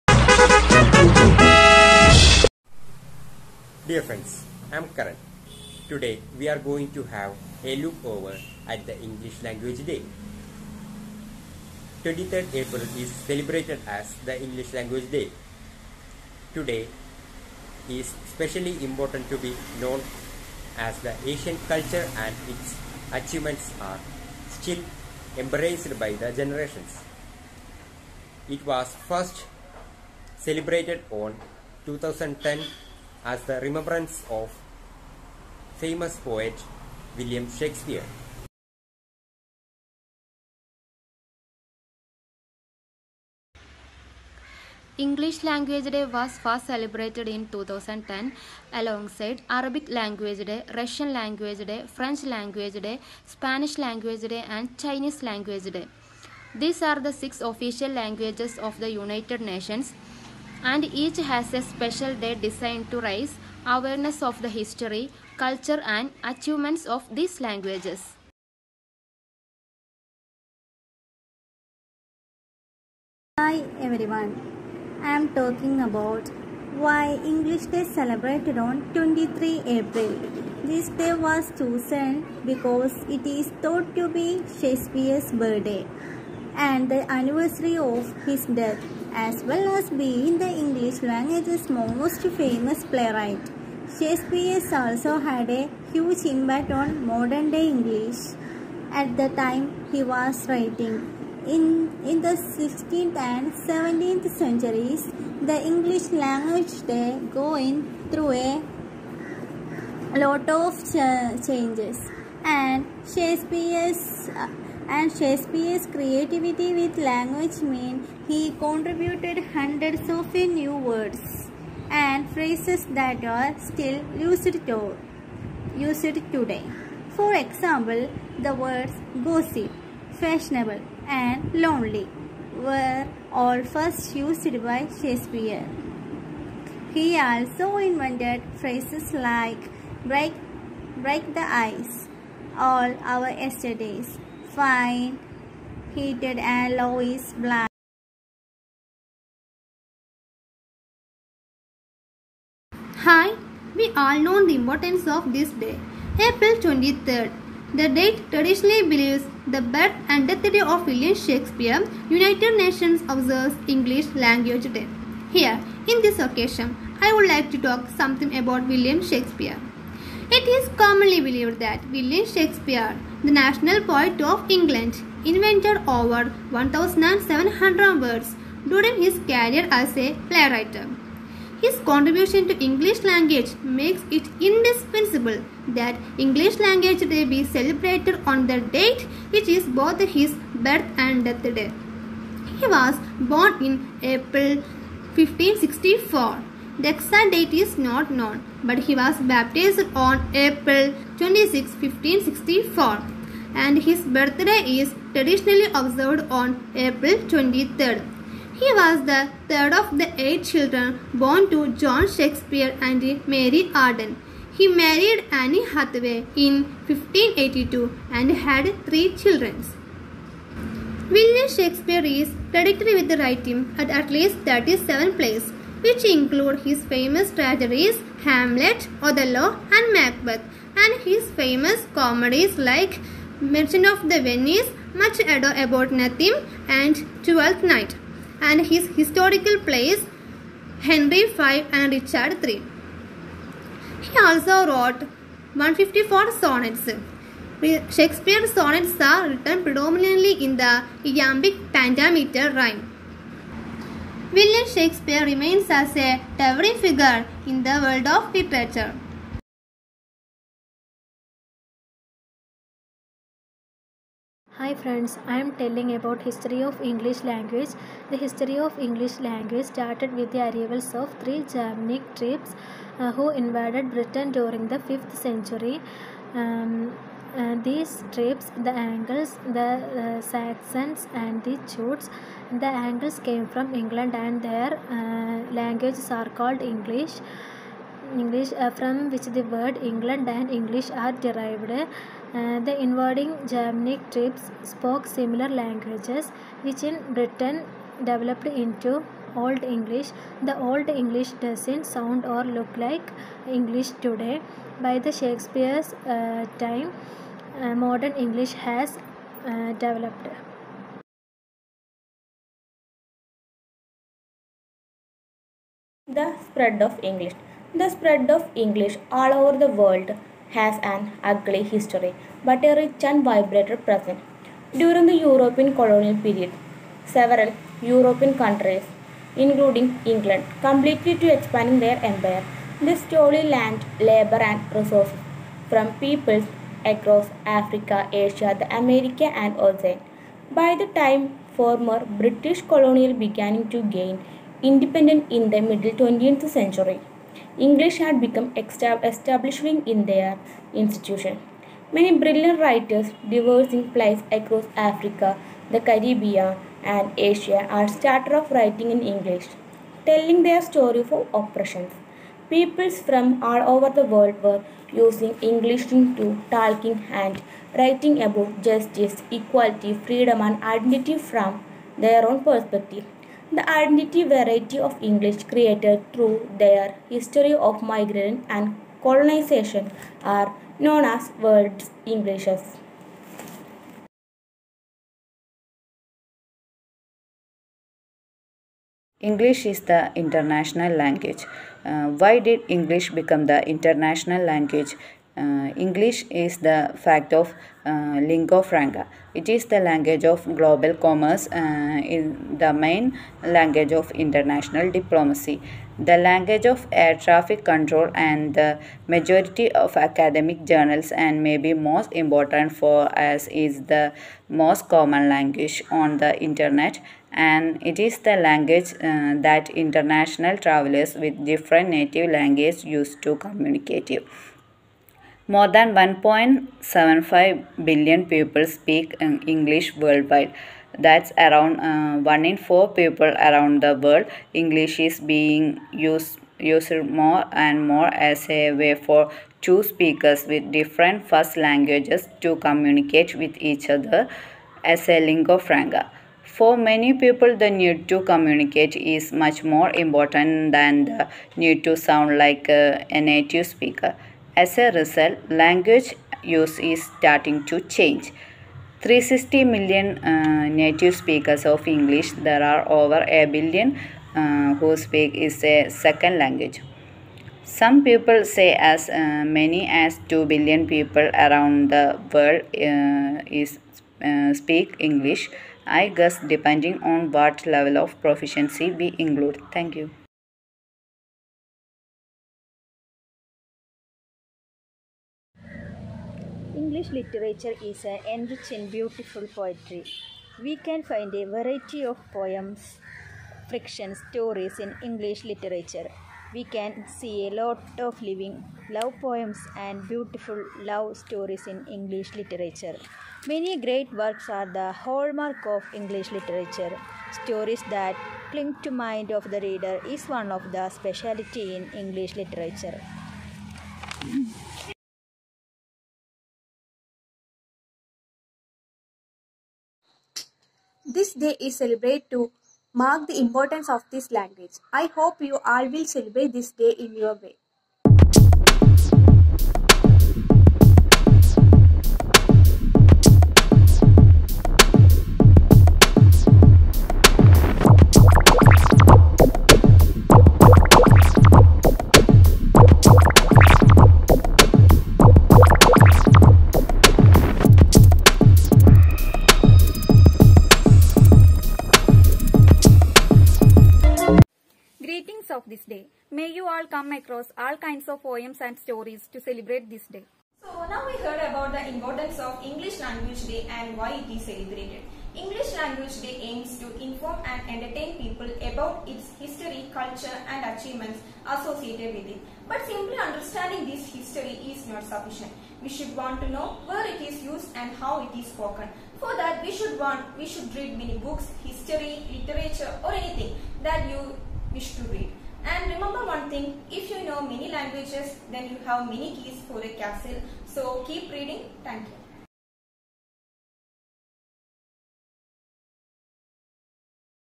English language day celebration by SNTCE STUDENTS.
Celebration of world English day on April 23rd by D.EL.ED  course candidates.
(LOCK DOWN CHALLENGE)